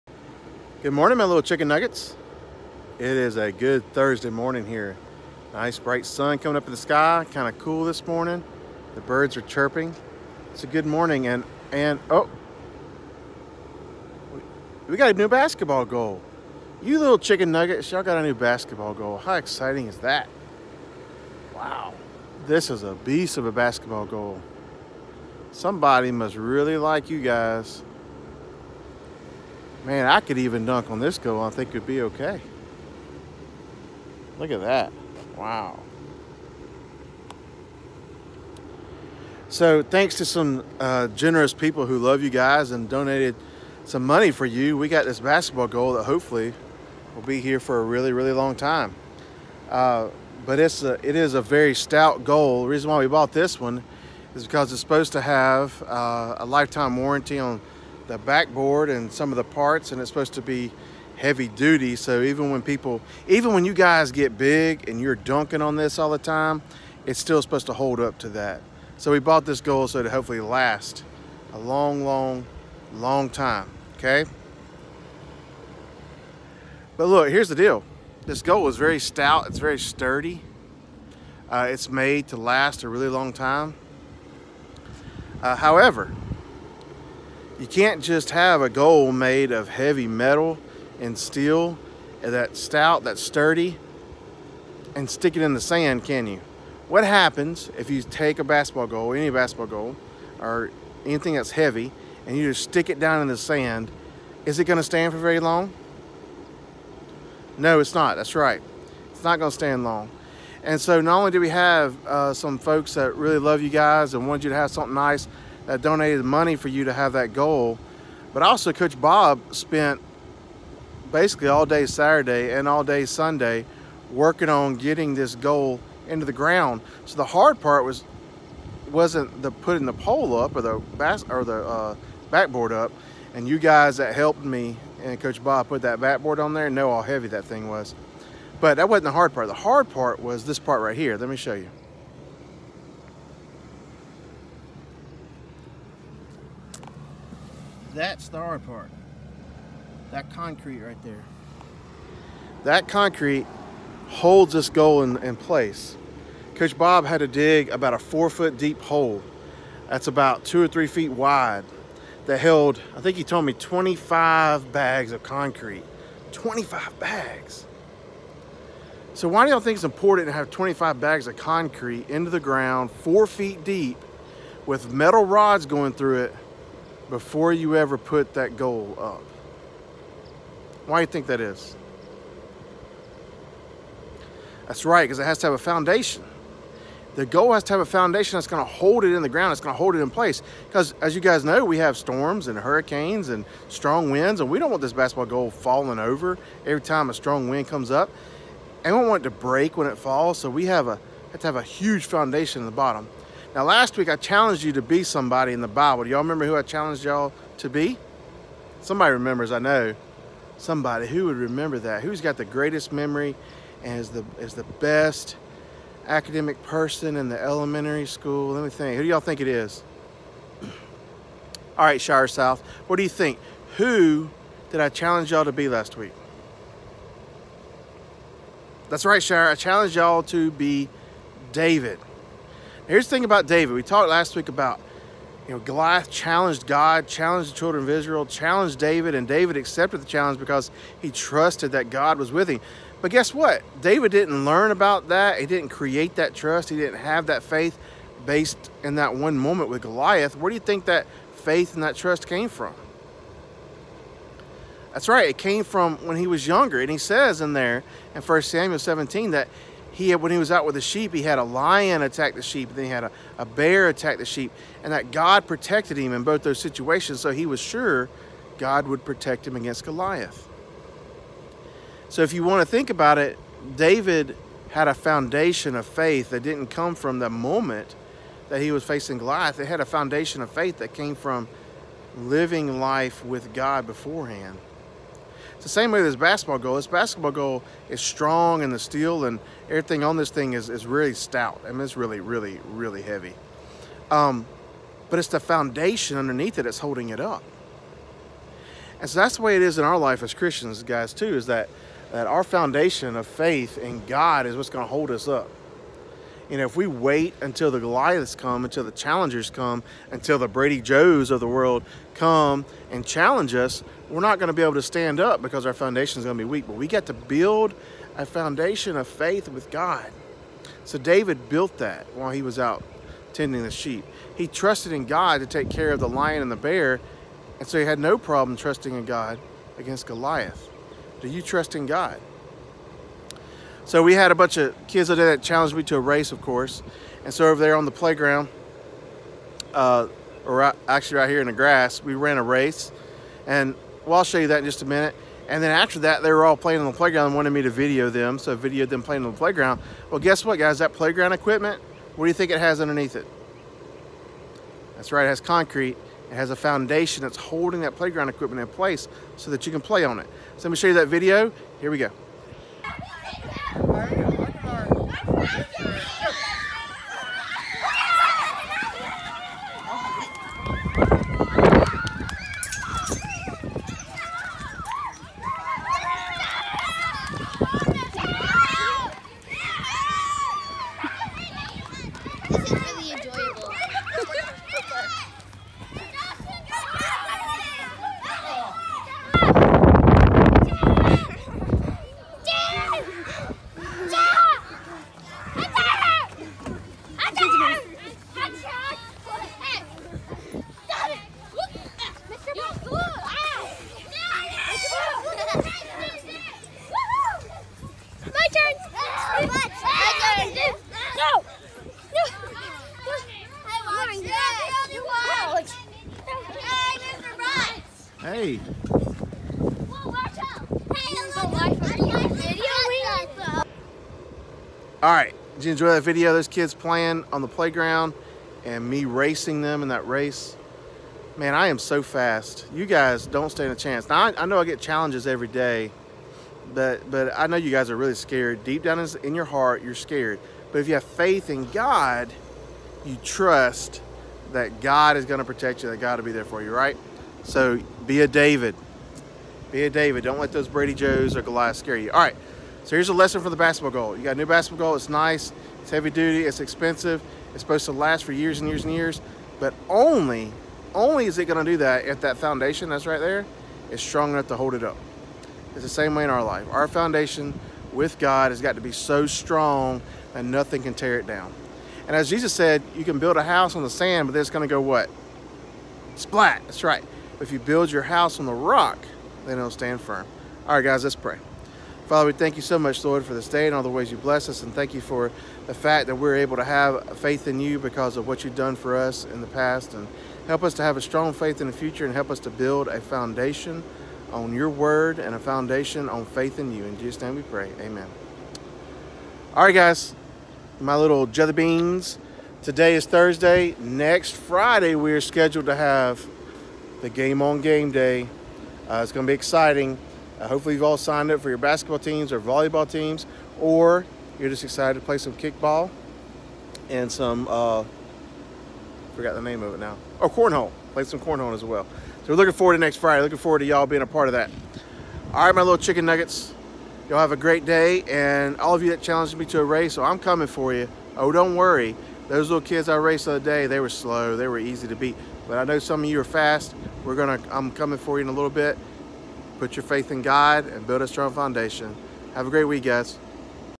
New Basketball Goal! Mr. Butts races 2nd Graders.
SCA Chapel 10-22-20 A new basketball goal needs a good foundation, just like we need in our Christian lives.  David stood up to Goliath because he had a foundation of faith that was established before he met Goliath.  Mr. Butts races the 2nd grade class and videos them having fun on the playground.